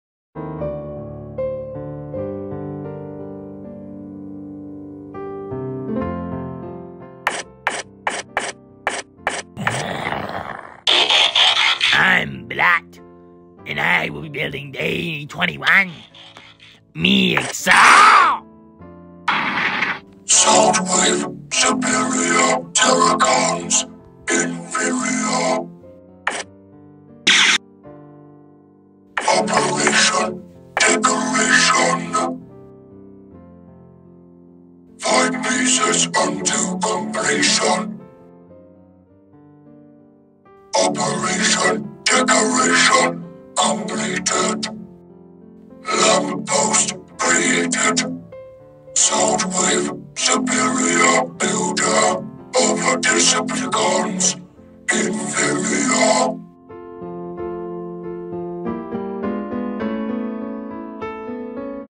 Transformers Christmas 🎄 Day 21 w/ Blot (Stop Motion)
Blot continues Christmas with Day 21 of the Advent Calendar